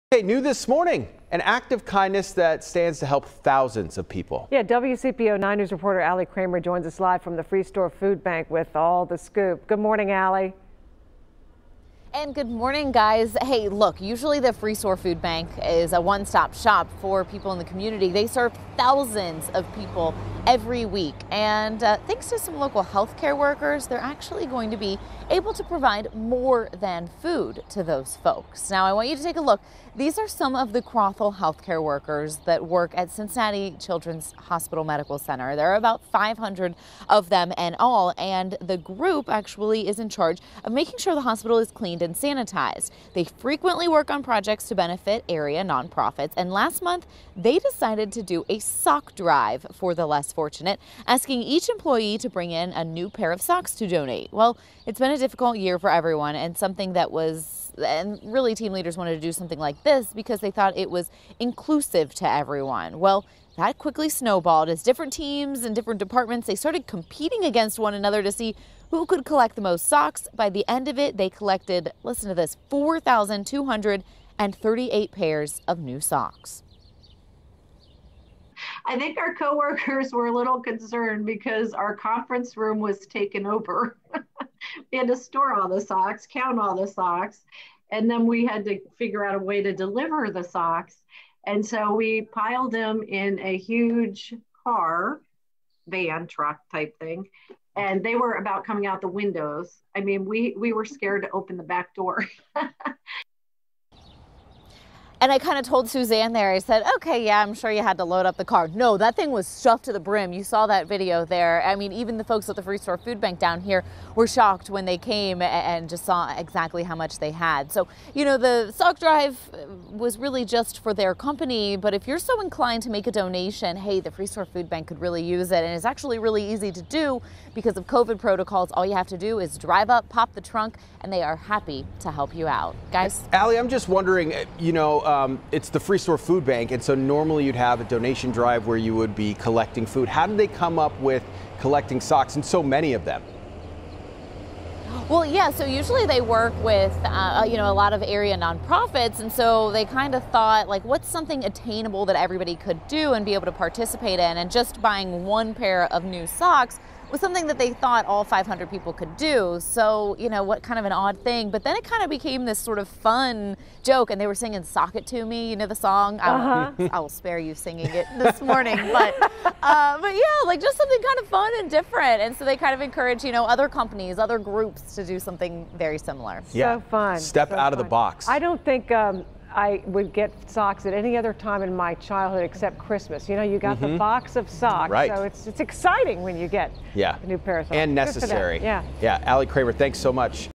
Healthcare workers decide to 'Sock it to' the Freestore Foodbank
Some healthcare workers are working with the Freestore Foodbank to ensure they will have more than just food to give to those in need.